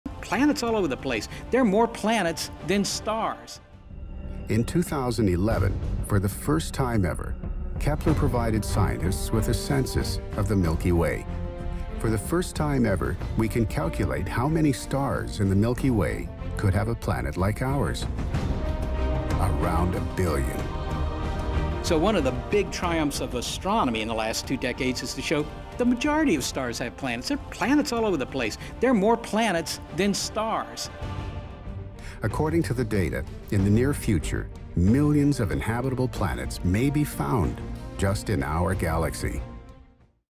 Could There Be Life On Other Planets?
With the massive amount of planets in our galaxy, the possibilities for new life is shockingly high.